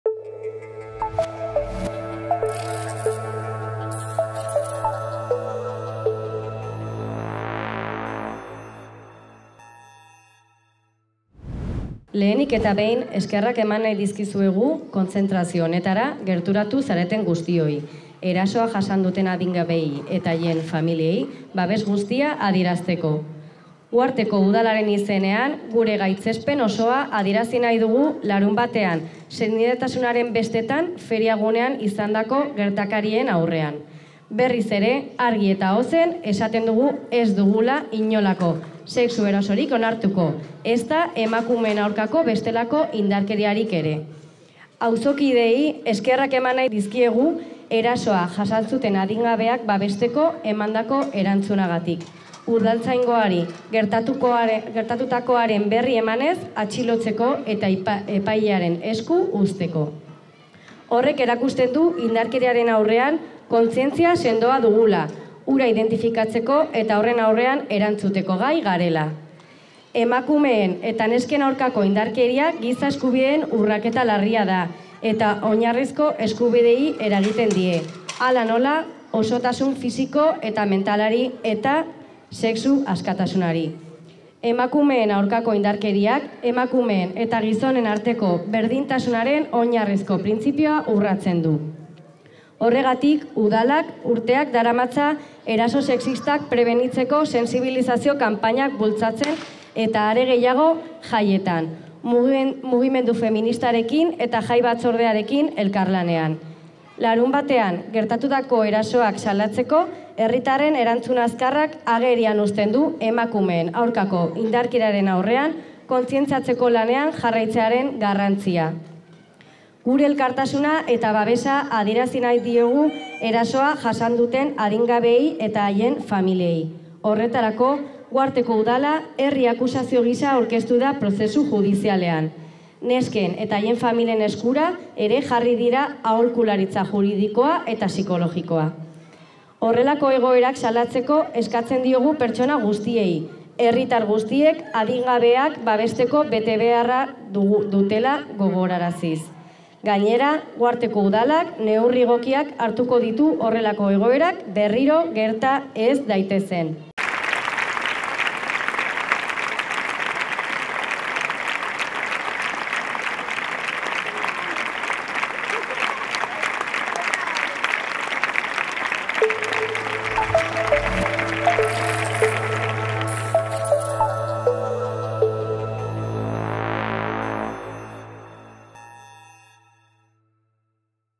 Uhartek larunbatean izandako erasoak gaitzetsi ditu
Uharteko Talde Feministak eta Udalak elkarrekin deitutako elkarretaratze batean hainbat adingabek euren jaietan jasandako sexu-erasoak salatu dira. Kaltetuei elkartasuna adierazi nahi izan diete, eta iragarri dute udala herri-akusazio gisa aurkeztuko dela prozesu judizialean. Era berean, mundu guztiari eskatzen diote horrelako egoerak salatzeko, eta iragarri dute Udalak neurri egokiak hartuko dituela horrelako gertaerak berriro gerta ez daitezen.